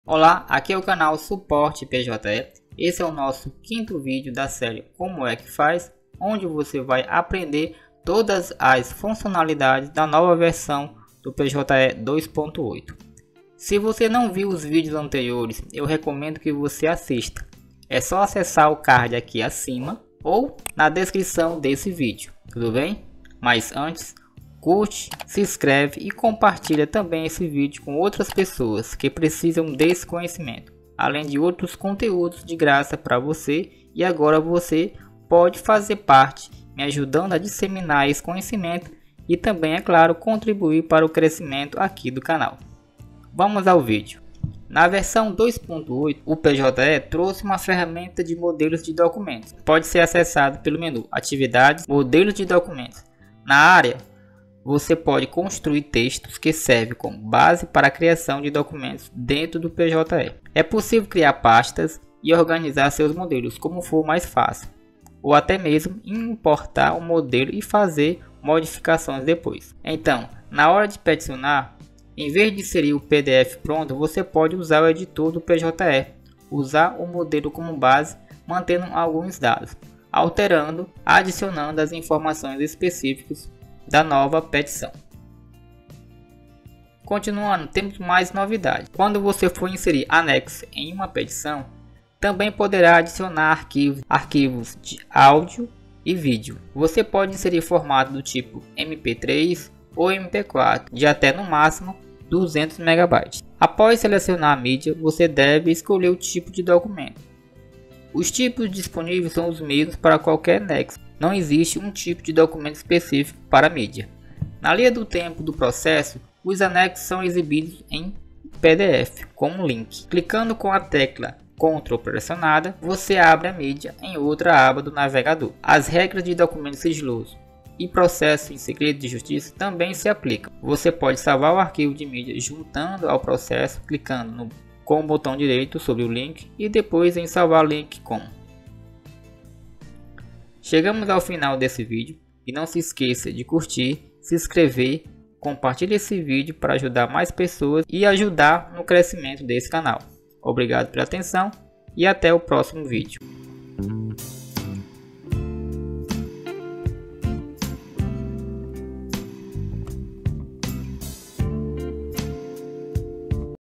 ⚖COMO É QUE FAZ – PJE | Modelos de Documentos Sistema PJe 2.8👈
🔥Nesse vídeo da “série como é que faz” você vai aprender como editar no modelos de documentos no sistema PJe 2.8. Torne-se um profissional especializado | Tutorial do Sistema PJe 2.8🔥

🔥Precisa de Suporte Remoto? Me chame no Whhatsapp!🔥